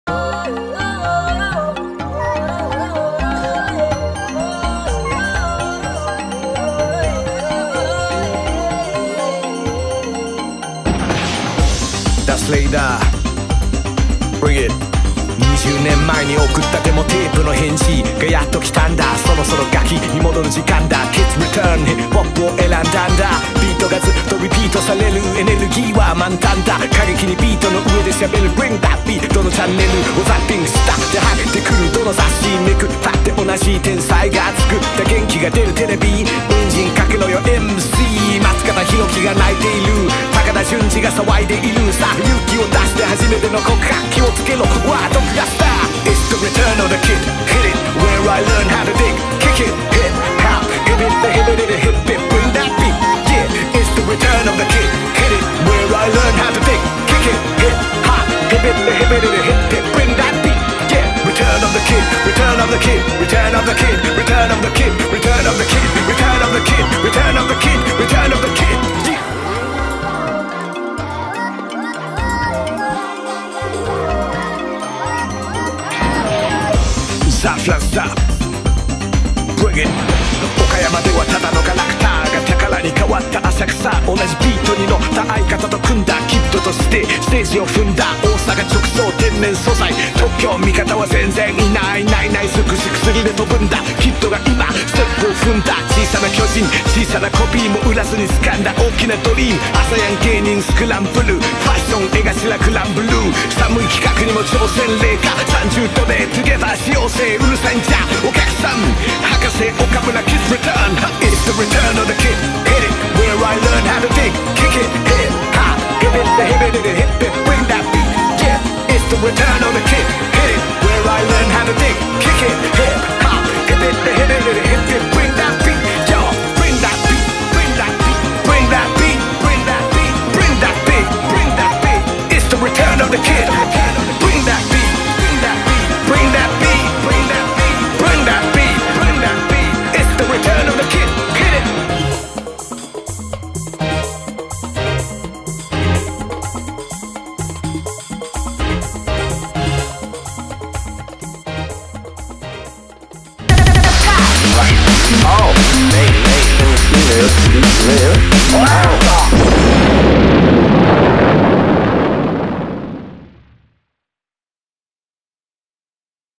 水道橋博士トークショー ザ☆フランス座5オープニング
水道橋博士のトークライブのオープニング映像
ラッパーのダースレイダーさんによるラップがカッコいい。

水道橋博士トークショウ　ザ☆フランス座Tシャツ